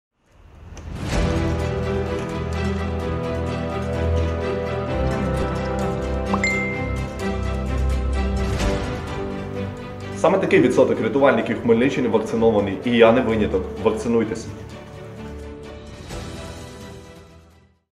Щеплення від COVID-19 рятують життя!
COVID-19 обмежив нашу свободу. Крім того, страшна хвороба продовжує забирати життя наших рідних та близьких. Рятувальники ДСНС своїм прикладом закликають українців вакцинуватися, аби мати можливість жити і працювати без карантинних обмежень. З цією метою в усіх територіальних підрозділах Служби порятунку розпочалася інформаційна кампанія «Ми вакциновані!».
На сьогодні вже більше 70% рятувальників та працівників ДСНС отримали обидві дози вакцини і збільшили свої шанси перенести хворобу у більш легкій формі, або ж взагалі уберегтися від неї. Саме тому ми нагадуємо: якщо ви так само прагнете повернутися до нормального життя, де немає страху за здоров’я своє та своїх рідних – вакцинуйтесь! Завітайте до найближчого пункту щеплень і не дайте пандемії шансів ще більше вкоренитися в наше сьогодення!
Пам’ятайте, кожна з вакцин від COVID-19 ефективно запобігає тяжкому перебігу хвороби й рятує життя.